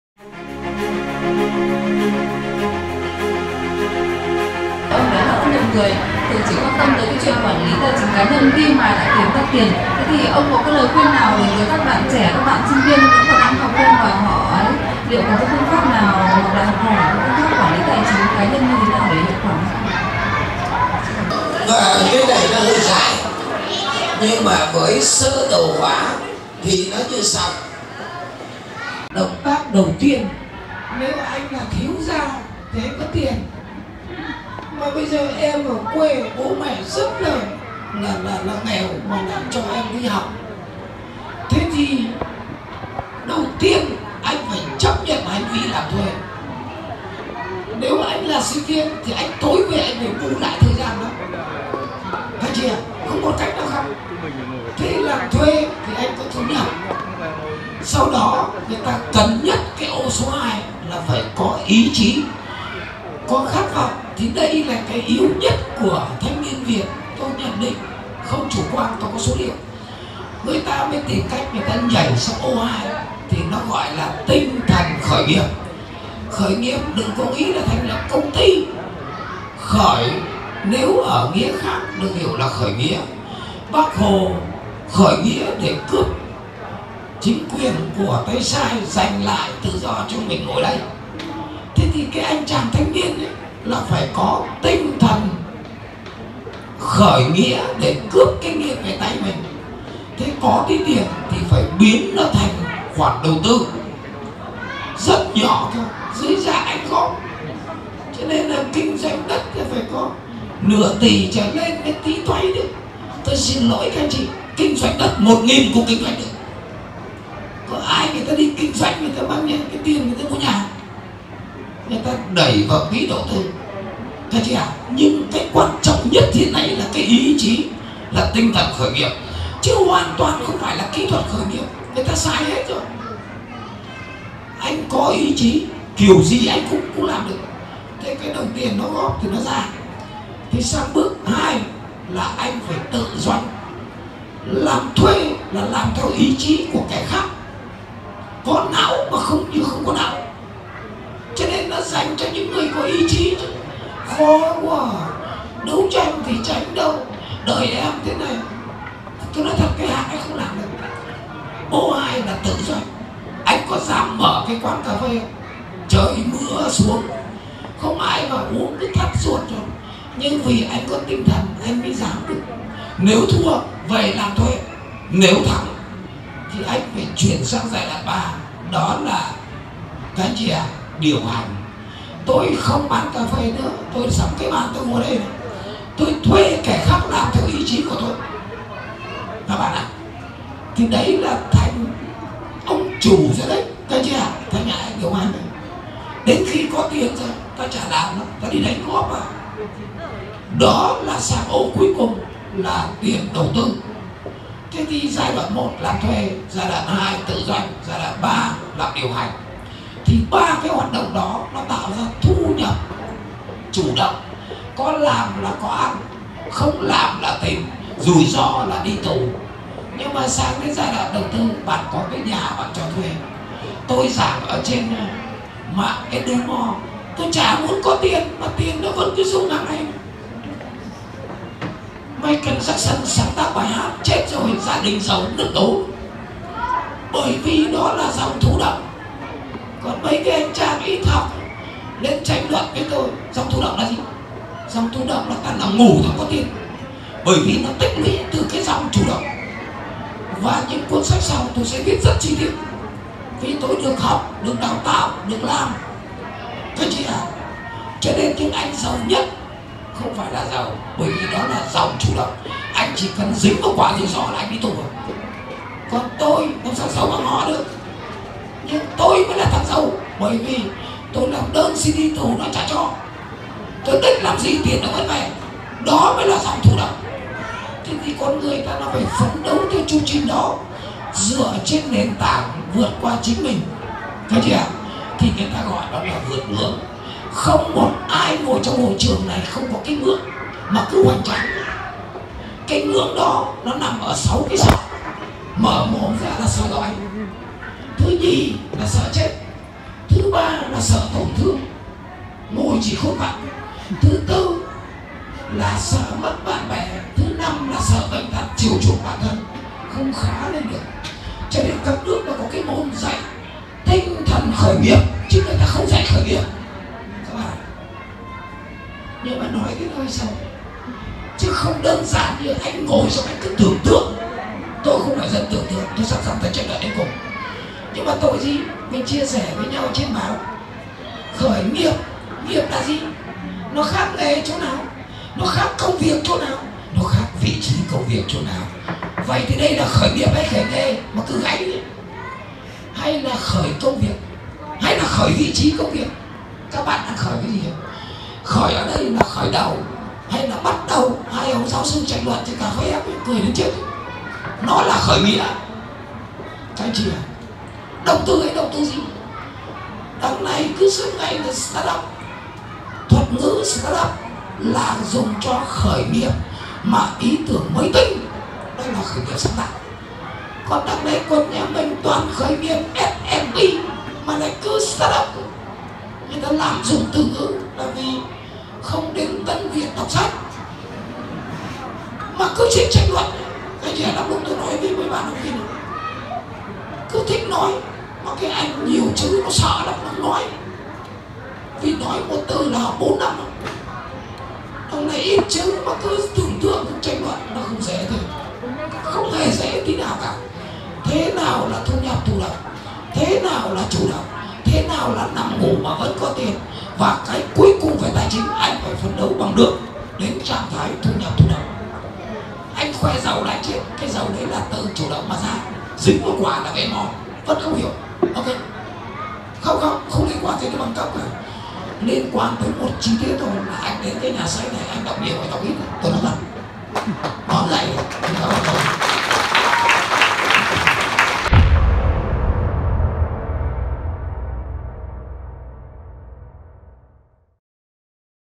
TS LÊ THẨM DƯƠNG: QUẢN LÝ TÀI CHÍNH CÁ NHÂN THẾ NÀO ĐỂ THÀNH CÔNG THỜI COVID-19?
Trích từ chương trình lễ ra mắt và ký tặng sách "Cảm xúc là kẻ thù số một của thành công" phiên bản 2021 được tổ chức tại TÂN VIỆT BOOKSTORE, VINCOM ROYAL CITY tối 13/3/2021.
Chương trình được tổ chức bởi hệ thống nhà sách Tân Việt và CLAZO - thương hiệu viết sách cho người nổi tiếng.
Sách "Cảm xúc là kẻ thù số một của thành công" phiên bản 2021 của TS Lê Thẩm Dương được chấp bút bởi nhà báo Nguyễn Tuấn Anh được phát hành tại các nhà sách uy tín: Tân Việt, Fahasa, Phương Nam, Tiki, Shopee.